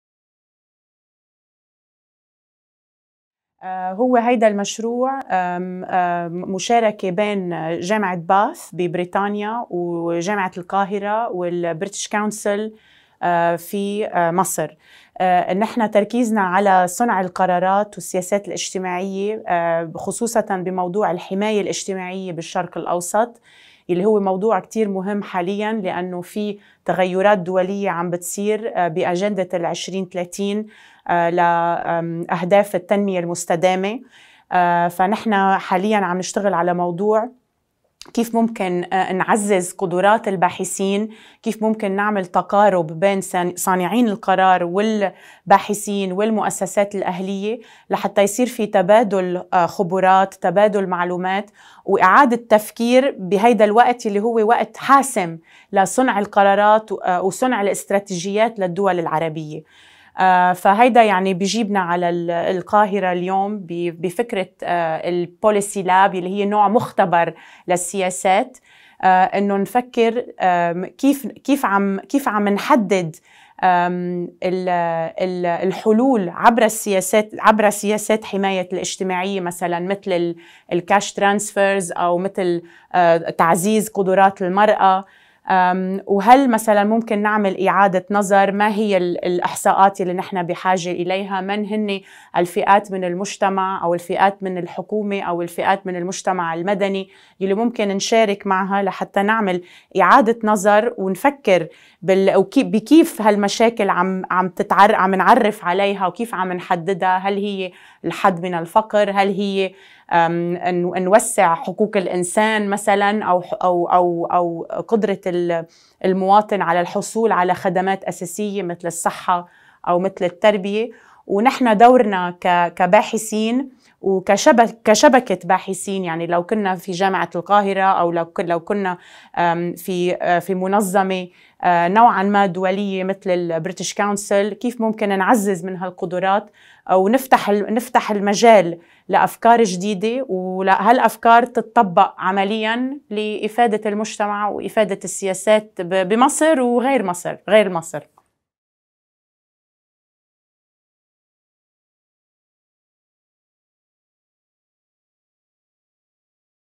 MENA Social Policy Network Dr Rana Jawad Interview (Arabic)
Interview with Dr Rana Jawad, senior lecturer at the University of Bath and MENA Social Policy Network founder. Filmed at Cairo University in April 2019.